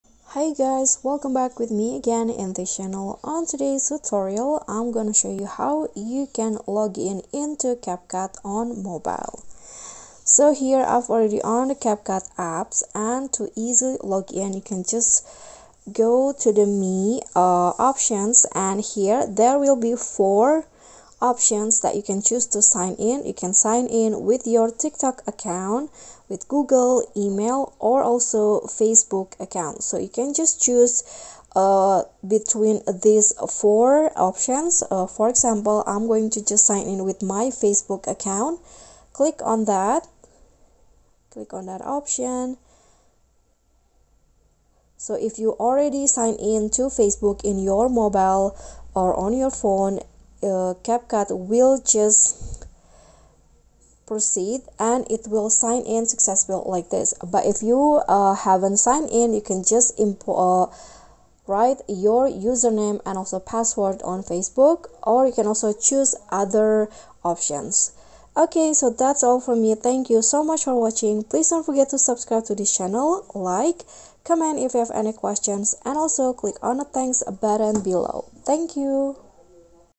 How To Login Into CapCut on Mobile? NEW UPDATE 2023 | CapCut Basic Tutorial for Beginners
This is how you can log in to CapCut on mobile